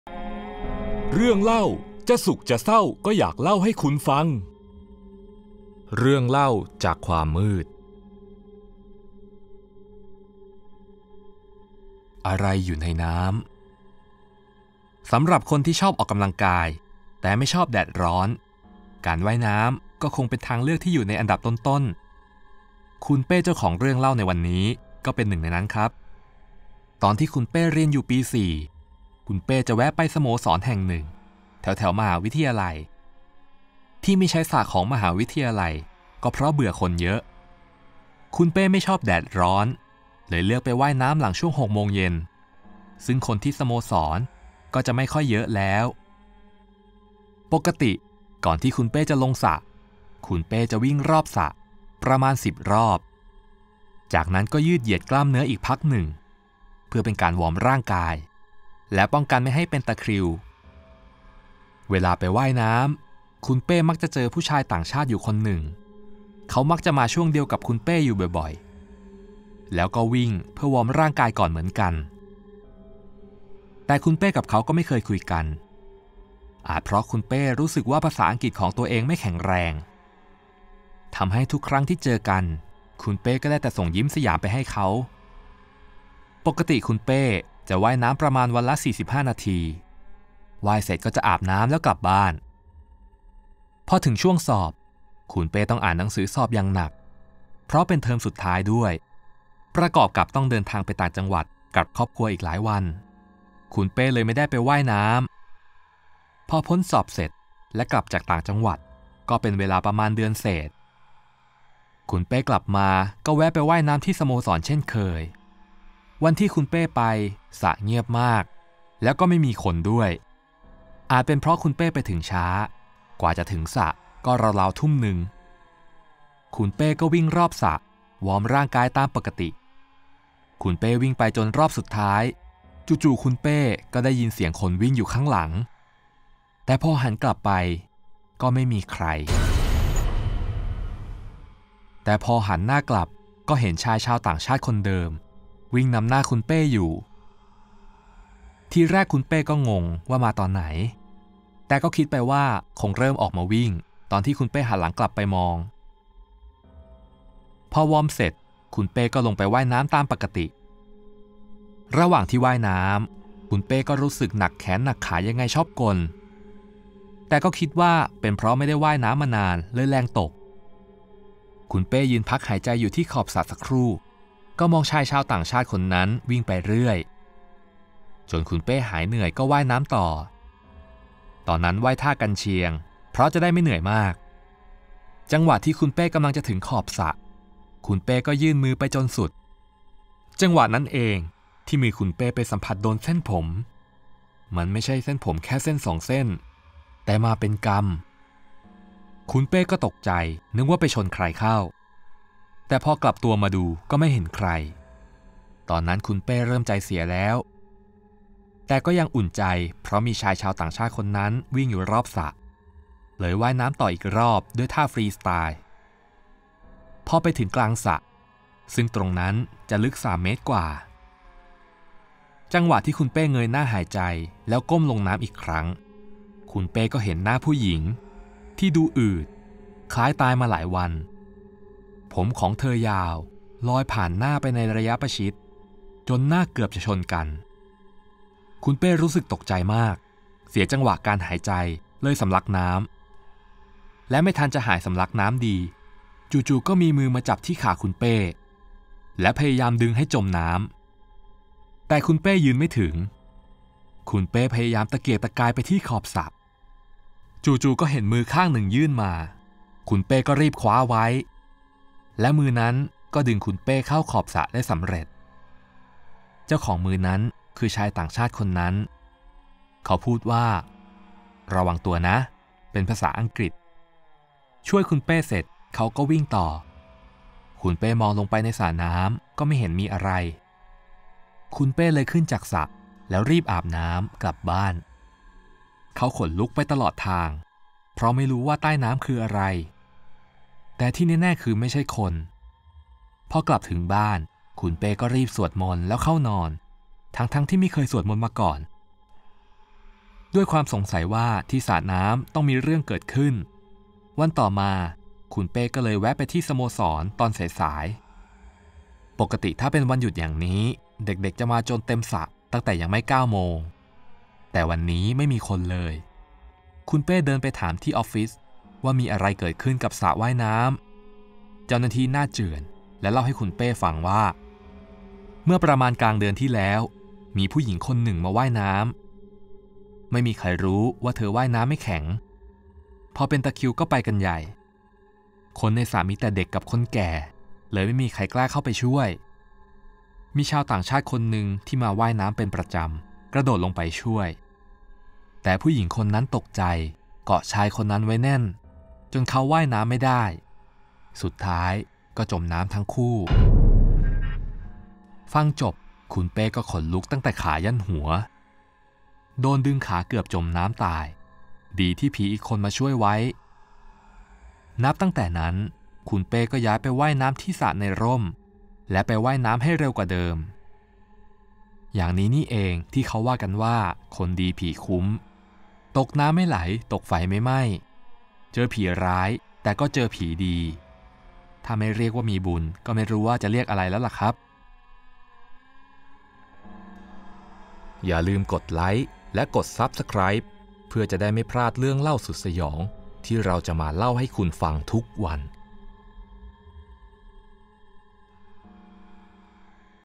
อะไรอยู่ในน้ำ l เรื่องเล่า
... คุณเป้ยืนพักหายใจที่ขอบสระสักครู่ ก็มองชายต่างชาติคนนั้นวิ่งไปเรื่อย จนคุณเป้หายเหนื่อย      ก็ว่ายต่อ ตอนนั้นว่ายท่ากรรเชียง เพราะจะได้ไม่ค่อยเหนื่อยมมาก จังหวะที่คุณเป้กำลังจะถึงขอบสระ คุณเป้ก็ยื่นมือไปจนสุด จังหวะนั้นเองที่มือคุณเป้ไปสัมผัสโดนเส้นผม มันไม่ใช่แค่เส้นสองเส้น แต่มาเป็นกำ   คุณเป้ก็ตกใจนึกว่าไปชนใครเข้า แต่พอกลับตัวมาดูก็ไม่เห็นใคร ตอนนั้นคุณเป้เริ่มใจเสียแล้ว ...